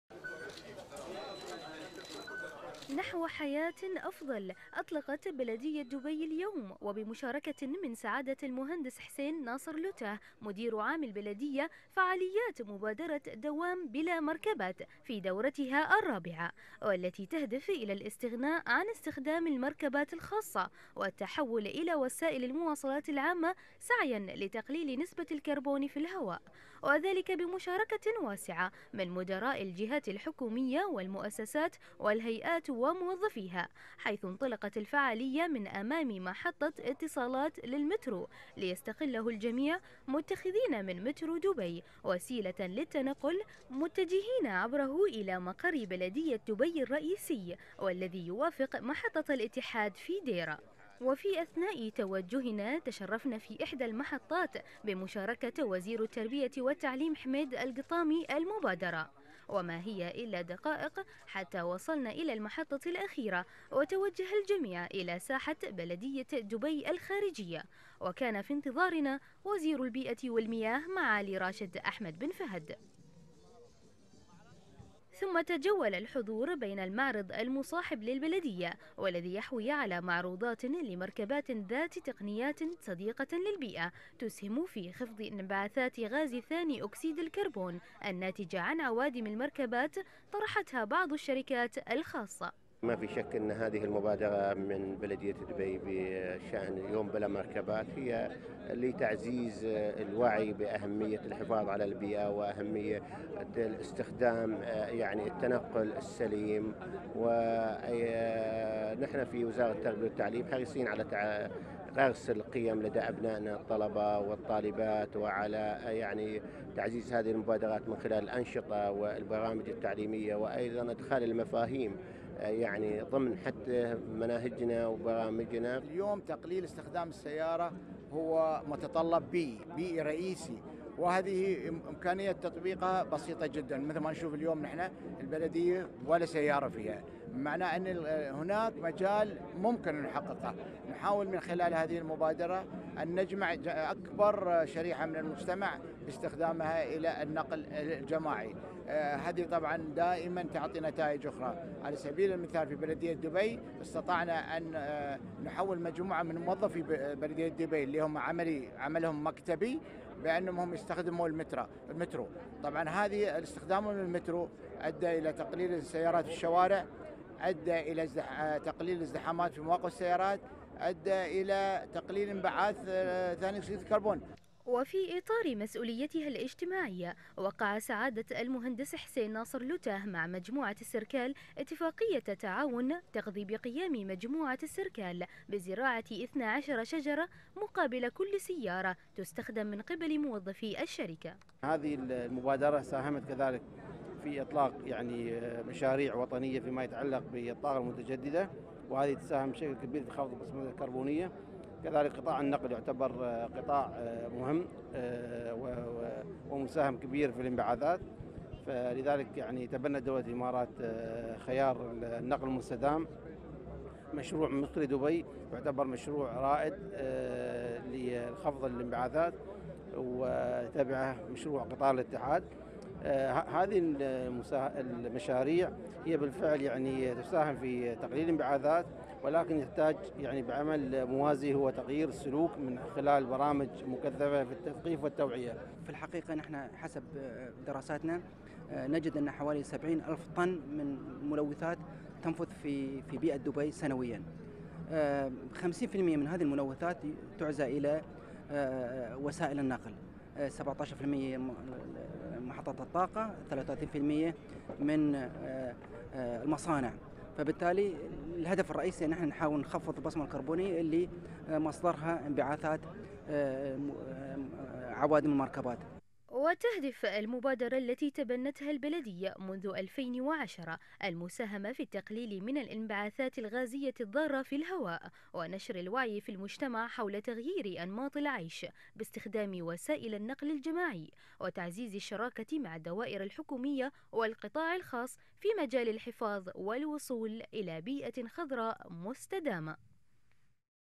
Car Free Day Dubai Dubai Media 13 02 20132
مشاركة وزارة البيئة و المياه في "يوم بلا مركبات"

تغطية تلفزيون دبي ليوم بلا مركبات